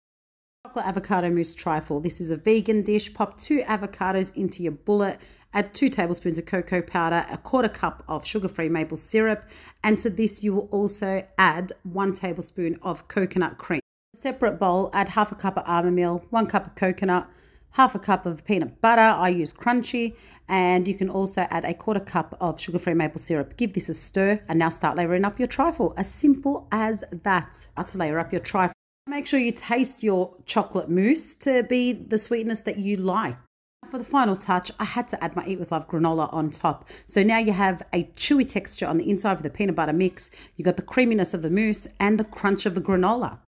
Keto Vegan Chocolate Avocado Mouse Trifle
Vegan Chocolate Avocado Mouse Trifle.

 Quick and easy to make using basic ingredients, Keto Friendly, Low Carb, Refined Sugar Free, Gluten Free, Vegan.

Recipe below 👇

Place in your bullet or blender
2 whole Avocado
2 tbs coco powder
1/4 cup of sugar free maple syrup
1 tbs coconut cream

Give it a blend and taste to adjust sweetness to your liking.

In a separate bowl add

1/2 cup of Blanched Almond Meal
1 cup Desiccated coconut
1/2 cup of crunchy peanut butter
1/4 cup of sugar free maple syrup

Mix and now assemble your trifle.

Final touch add your Eat With Love Granola on top for the crunch and enjoy 😉